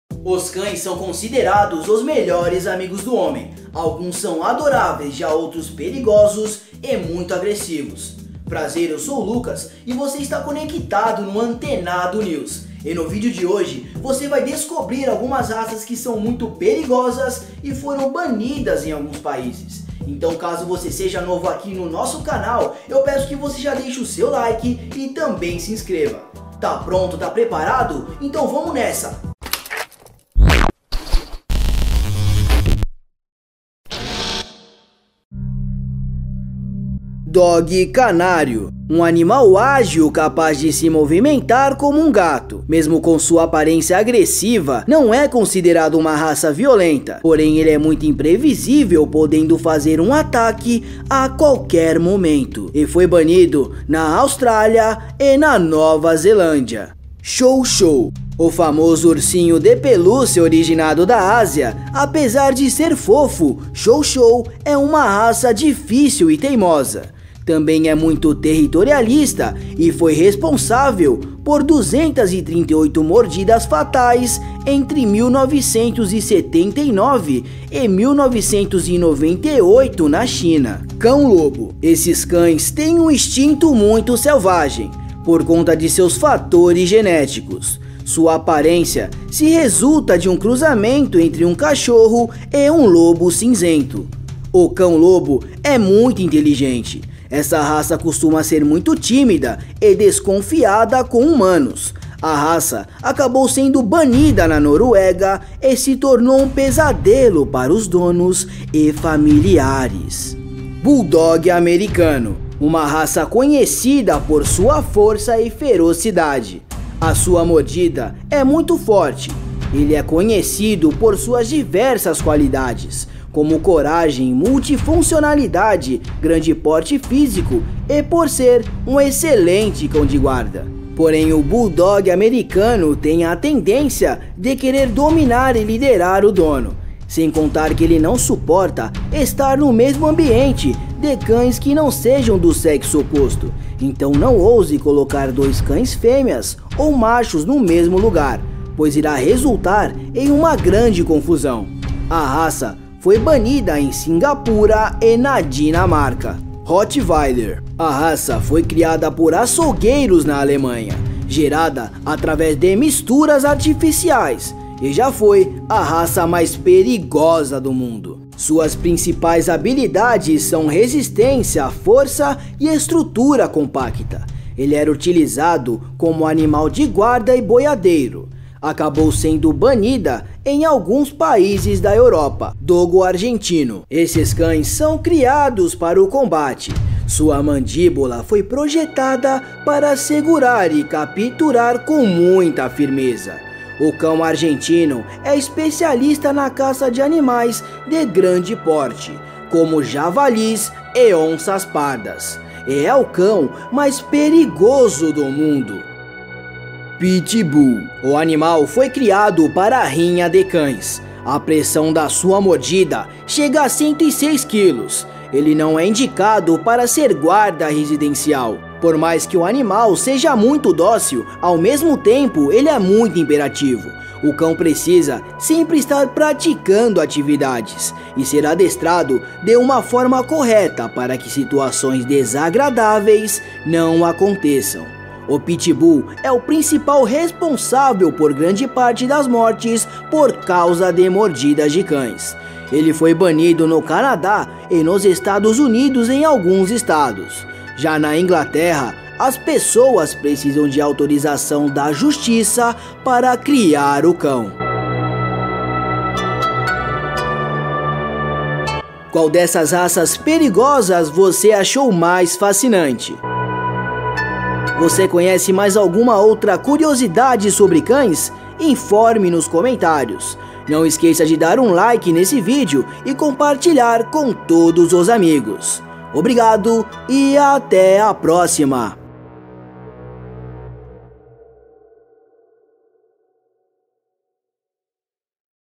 As 7 Raças de Cães com as Mordidas mais fortes e Mortais do Mundo
Raça dos cães proibidos e perigosos do mundo

THE 7 MOST DANGEROUS DOGS IN THE WORLD

Sem dúvidas os cachorros são os melhores amigos do homem, além de ser fofos são ótimos companheiros, porém algumas raças quando não são adestradas ou criadas corretamente acabam se tornando muito perigosas.  Hoje você vai ver as 7 raças dos cães mais perigosos do mundo, que foram banidos em alguns países.

TOP 7 : Dogue Canário, Chow Chow, Wolfdog, Bulldog Americano, ROTTWILER, Dogo Argentino, PitBull.